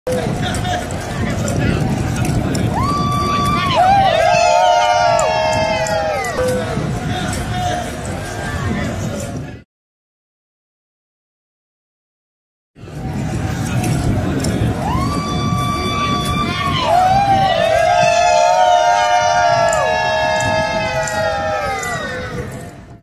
Man runs in green speedo for Boston's 2015 santa race
cheerleaders